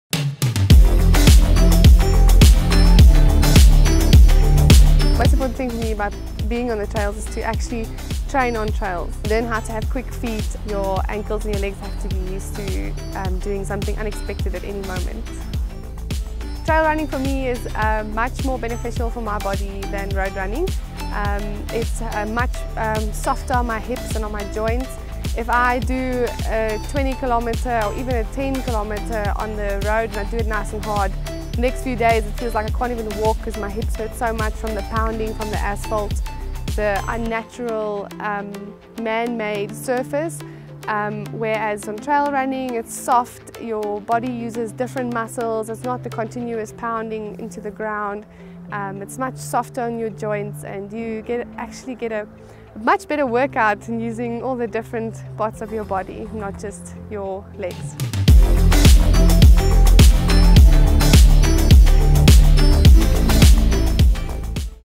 Trail running tips from Jani Du Toit
In this episode of All Out Trail: Jani du Toit from Asics Frontrunners share her top tips for families who would like to start running and how to ease into trail running.